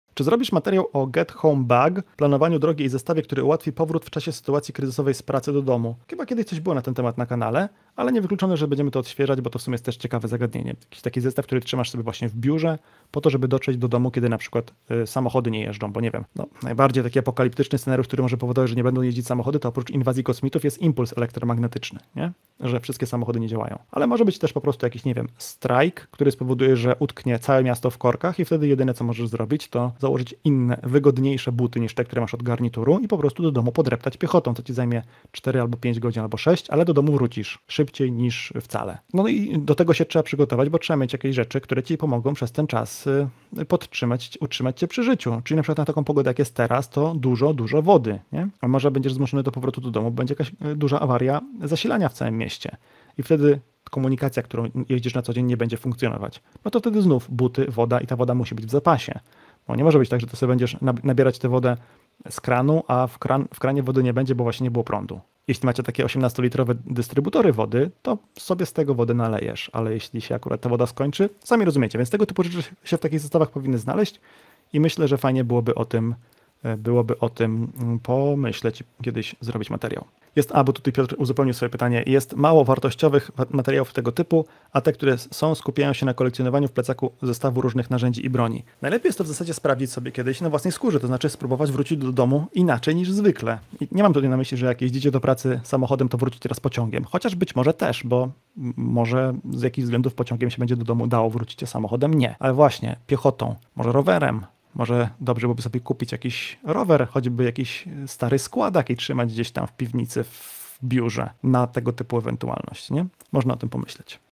Po co komu "get home bag"?
Czym jest "get home bag"? Po co komu taki zestaw przetrwania? O tym opowiem na tym krótkim, hehe, filmiku.

To fragment wycięty z ostatniej transmisji na żywo, którą w całości znajdziesz tutaj:

A w wersji do posłuchania w popularnych serwisach z podcastami oraz na blogu

Plecak na miniaturce dostaliśmy od Helikona.

Dotacje w Bitcoin przyjmujemy tu:
18Mk6j2mFcky1vJeiqxrZteEeuhthvRd9